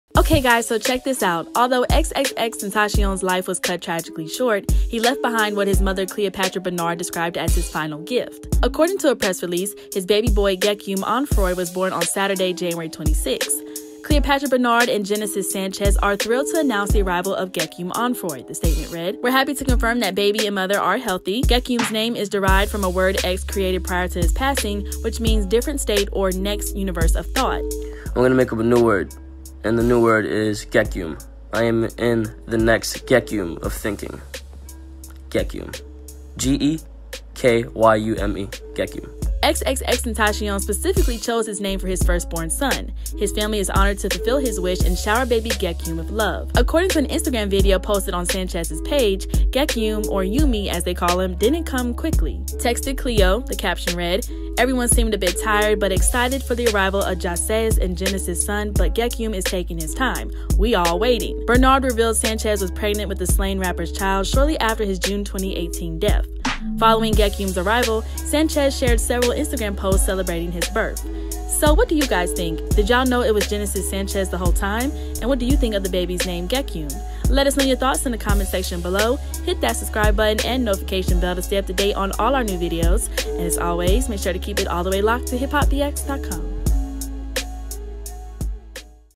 XXXTENTACION Mother Shares First Image Of His New Baby Son Gekyume
Although XXXTENTACION’s life was cut tragically short, he left behind what his mother Cleopatra Bernard described as his “final gift.” According to a press release, his baby boy Gekyume Onfroy was born on Saturday (January 26).